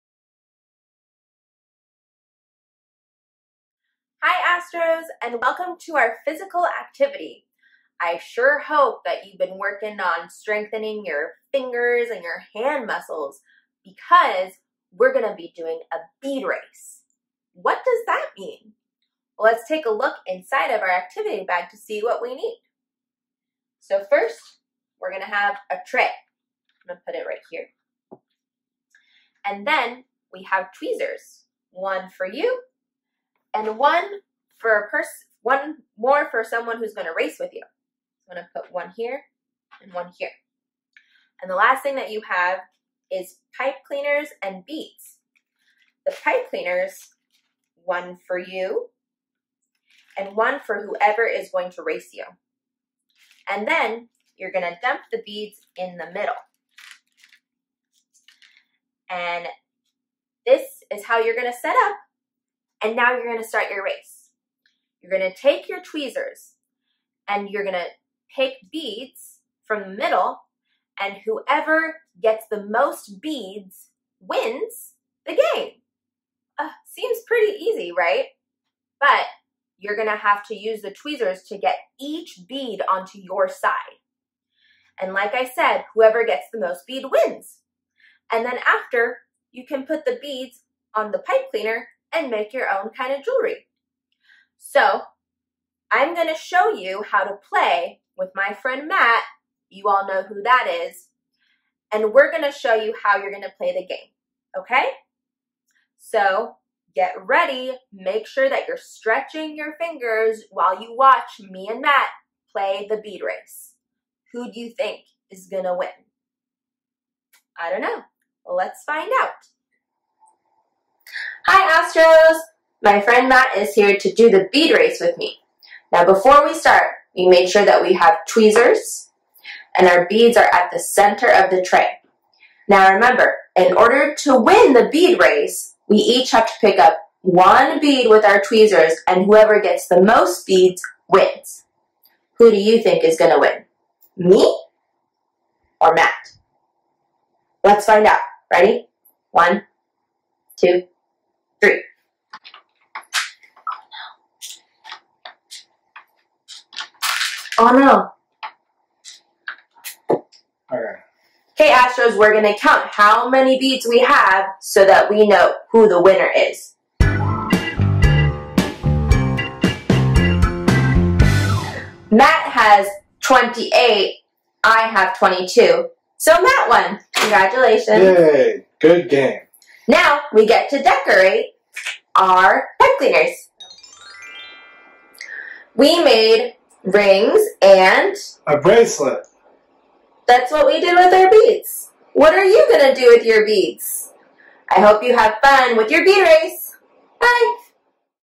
Bead Race
Have a bead race with Teacher Gigi and her friend, Matt!!